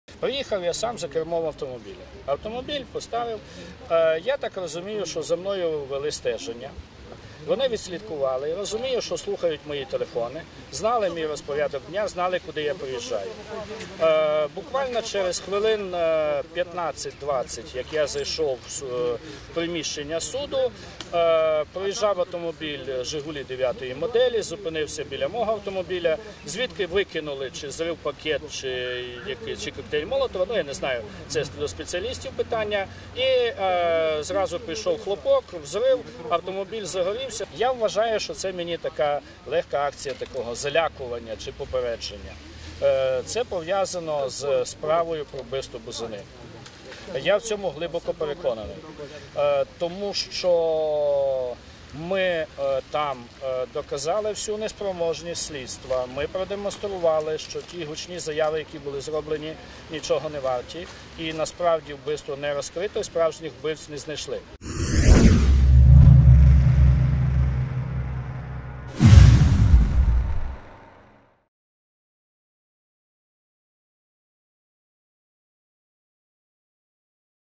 В Киеве взорвали авто адвоката по делу Бузины
Возле Соломенского районного суда Киева взорвали машину адвоката Андрея Федура, который выступает защитником подозреваемых в убийстве Олеся Бузины. По словам Федура, это - предупреждение ему или даже покушение на убийство. К моменту прибытия журналистов "17 канала" на место происшествия, там уже работали эксперты-взрывотехники.